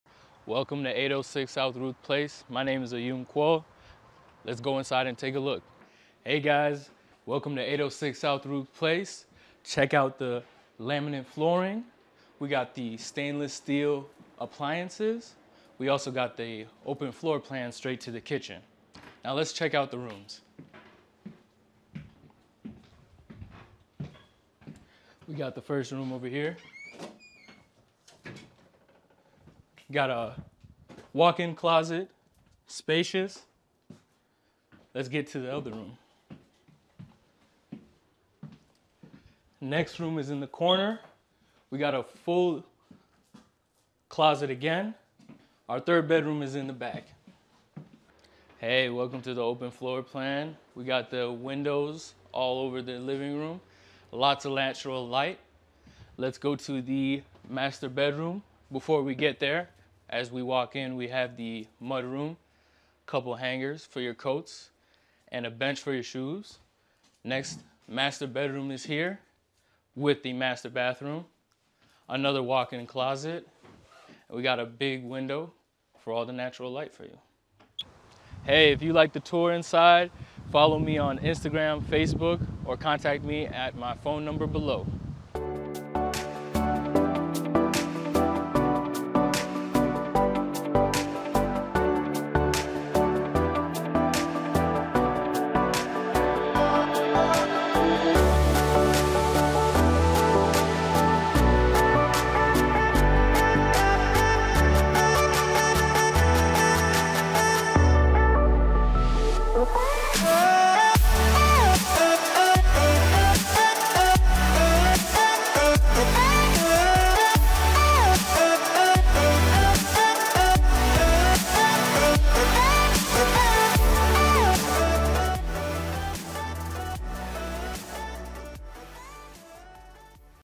Walk Through With Me | 806 S Ruth Ave, Sioux Falls SD
🏡 806 South Ruth Place, Sioux Falls, SD 57106 - $115,000

Welcome to your future home! This charming three-bedroom, two-bathroom residence is situated in the serene Marion Manor Mobile Park and offers modern comforts at an unbeatable price. Built in 2021, this 1,216 sqft home features contemporary appliances, including a brand-new washer and dryer, purchased within the last six months.

Key Features:

🏠 1,216 Square Feet of Living Space
🆕 Modern Appliances (New Washer & Dryer)
🛡️ Seven-Year Home Warranty
💰 $420 Monthly HOA Fee
Location Highlights:

🌳 Near Dunham Park with Scenic Trails
🛍️ Short Drive to Lake Lorraine Shopping & Restaurants
🚗 Convenient Access to Local Amenities
Explore the beauty and convenience of 806 South Ruth Place through our gallery and video tour. This home is perfect for families or individuals seeking a blend of modern living and community charm.

Presented by Ayum Kuol, Broker Associate

Consent is not a condition of purchase. Msg/data rates may apply. Msg frequency varies. Reply STOP to unsubscribe. [Privacy Policy / Terms & Conditions]